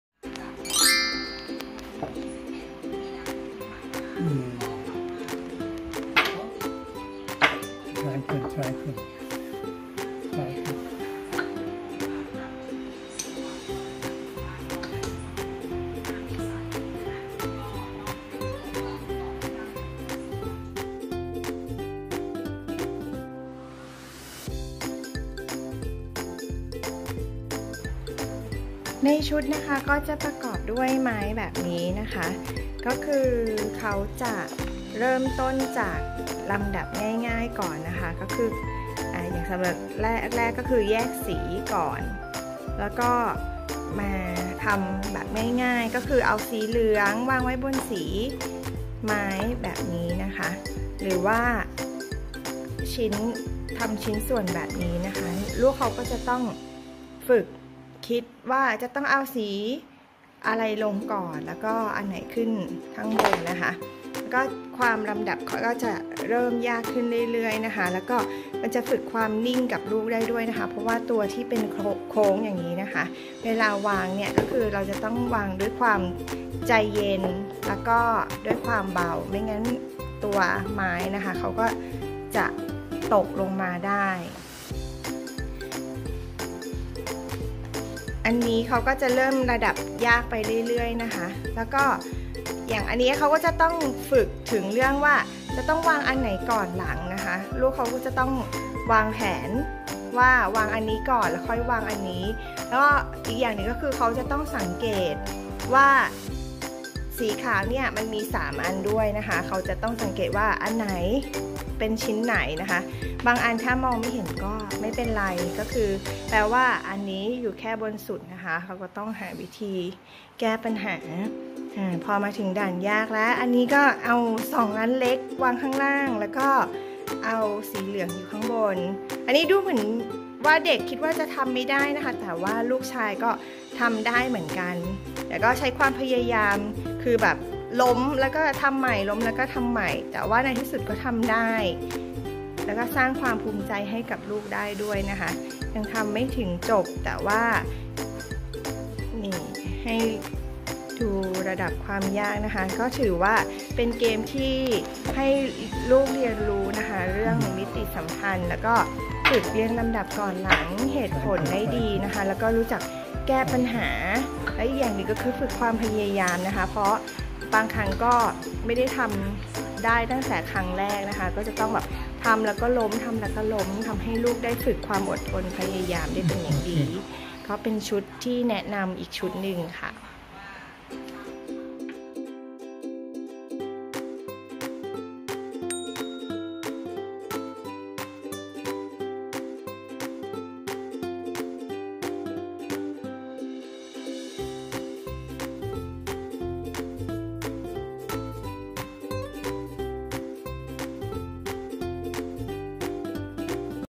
เกมพัฒนาสมองซีกซ้าย-ขวา STEM สำหรับ 3 - 8 ขวบ ชุด Building Blocks [Totty Books and Toys EP. 76]
เกมพัฒนาสมองซีกซ้าย-ขวา STEM สำหรับ 3 - 8 ขวบ ชุด Building Blocks

ฝึกความนิ่ง สมาธิ ความอดทน ความพยายาม EF รวมถึงมิติสัมพันธ์

วางชิ้นไม้ตามตำแหน่งที่กำหนด ฝึกการสังเกตุ การลำดับก่อนหลัง รวมถึงการวิเคราะห์แก้ปัญหา

การเรียนรู้ผ่านการเล่น สะสมเป็นความทรงจำระยะยาวทำให้เด็กสามารถรับรู้และเรียนรู้สิ่งรอบตัวไปตลอดชีวิต

ราคาอาจมีการเปลี่ยนแปลง โปรโมชั่นส่วนลด สอบถามได้ที่